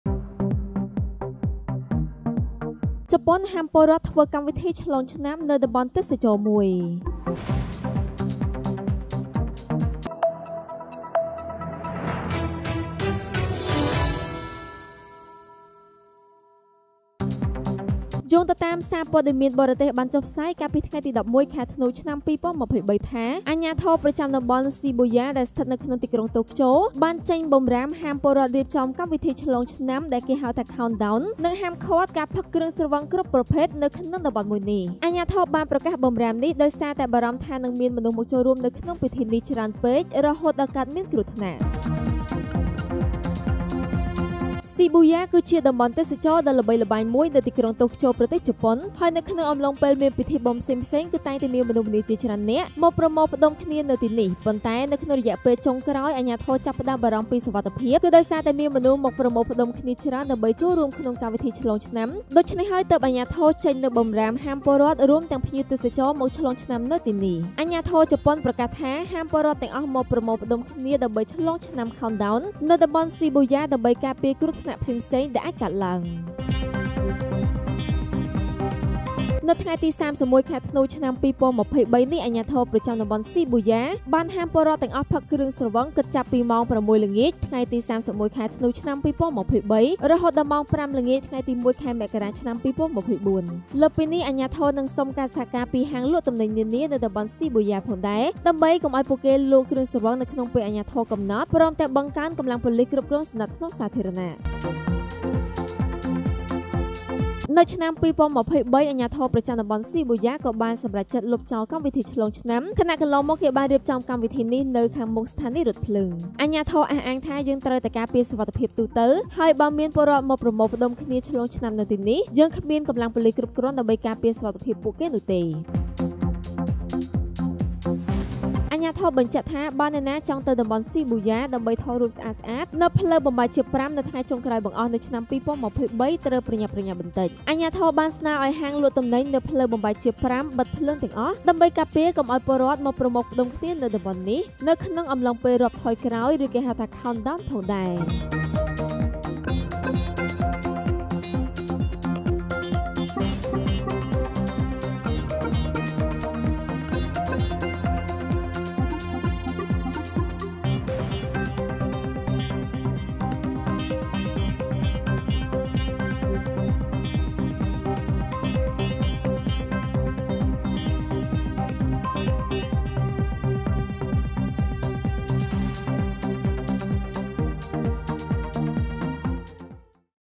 ជប៉ុនហាមពលរដ្ឋធ្វើកម្មវិធីឆ្លងឆ្នាំនៅតំបន់ទេសចរណ៍មួយ
ប្រទេសជប៉ុន ៖ យោងតាមសារព័ត៌មានបរទេសបានចេញផ្សាយ កាលពីថ្ងៃទី១១ ខែធ្នូ ឆ្នាំ២០២៣ ថា អាជ្ញាធរប្រចាំតំបន់ Shibuya ស្ថិតក្នុងទីក្រុងតូក្យូ បានចេញបម្រាមហាមពលរដ្ឋរៀបចំកម្មវិធីឆ្លងឆ្នាំ «Countdown» និងផឹកគ្រឿងស្រវឹងក្នុងតំបន់នេះ ។ អាជ្ញាធរបានប្រកាសបម្រាមនេះដោយសារបារម្ភថា នឹងមានមនុស្សមកចូលរួមក្នុងពិធីនេះច្រើនពេក រហូតដល់កើតគ្រោះថ្នាក់ ។